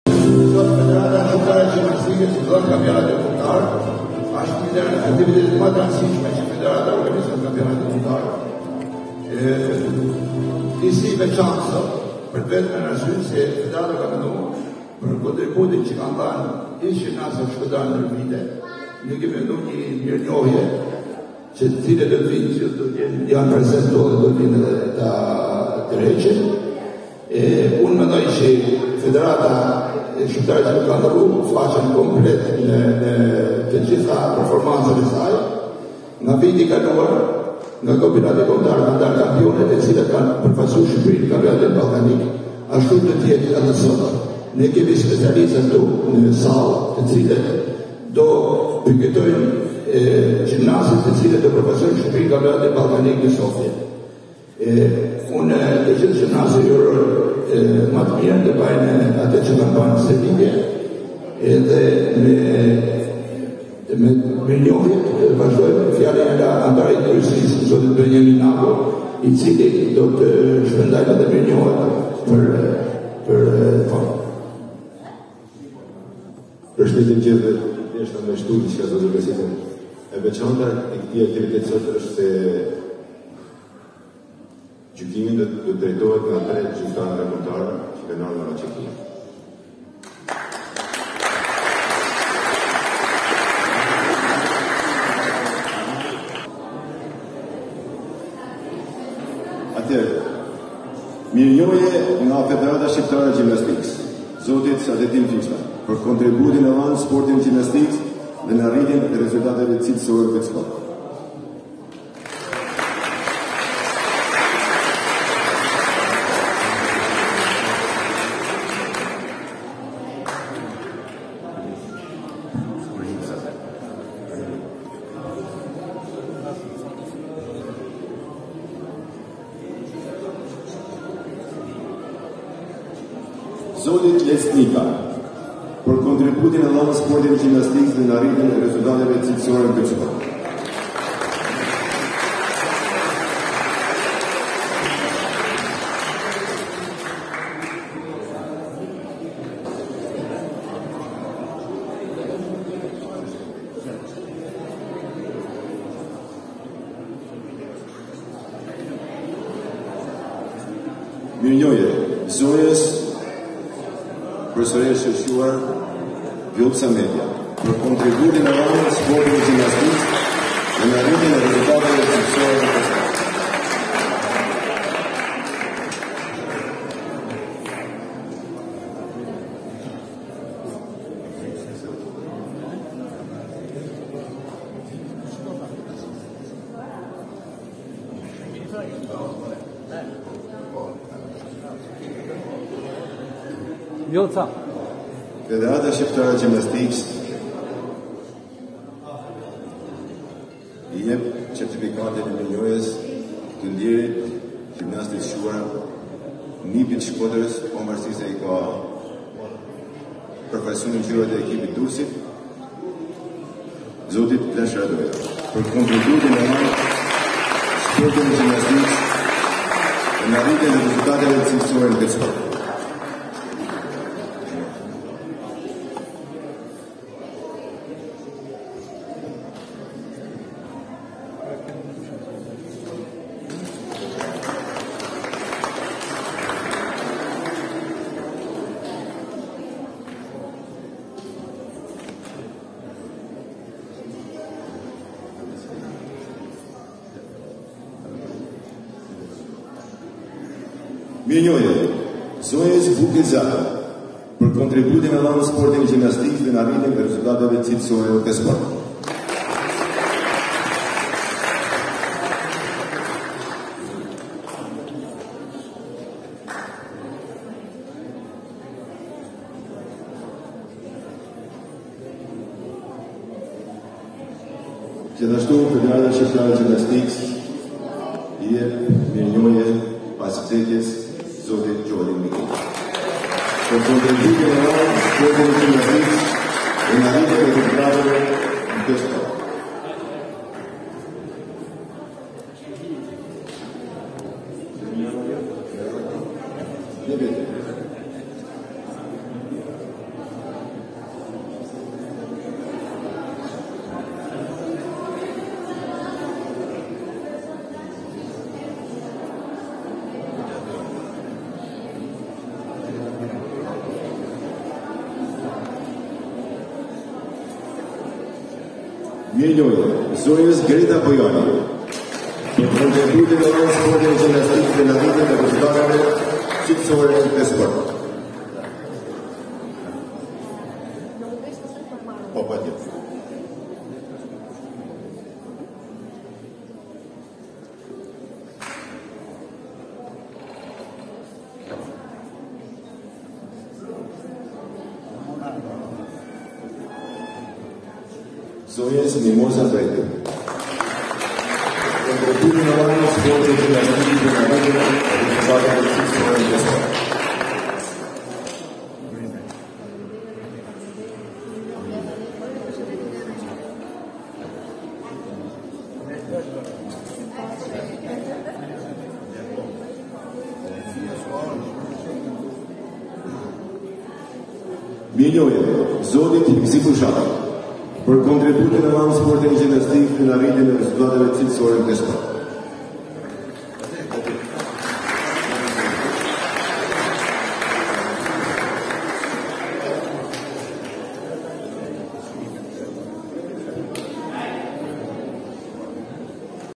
FSHGJ dekoron kampionët: Fishta, Media, Zako, Radoja, Mikeli, Tula, Bushati, Bregu, Bojani, Nika
Federata Shqiptare e Gjimnastikës dekoron 10 ish kampionë të Vllaznisë: Fishta, Media, Zako, Radoja, Mikeli, Tula, Bushati, Bregu, Bojani, Nika